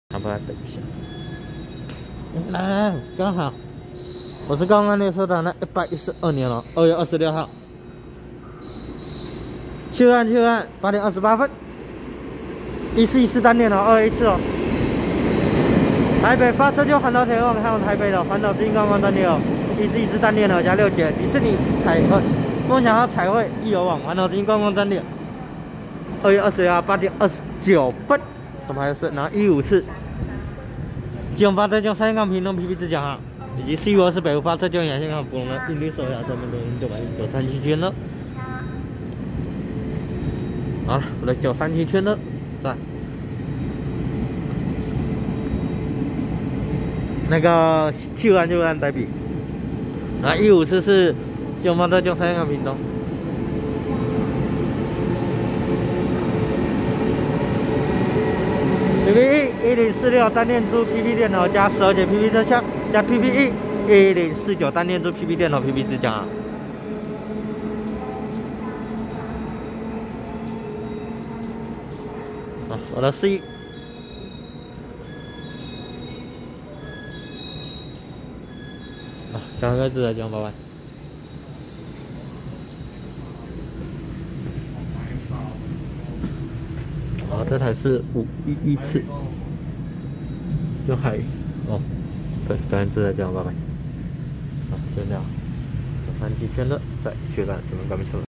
115次基隆發車經由山線開往屏東的PP自強號，2A次台北發車經由環島鐵路網開往台北的E414單電頭+六節迪士尼夢想號彩繪易遊網環島之星觀光專列
112/2/26七堵站8:28分拍攝4152次北湖發車經由宜蘭線開往福隆的銀綠色微笑號EMU900型十輛一編組EMU937區間車，115次基隆發車經由山線開往屏東的PP自強號，2A次台北發車經由環島鐵路網開往台北的E414單電頭+六節迪士尼夢想號彩繪易遊網環島之星觀光專列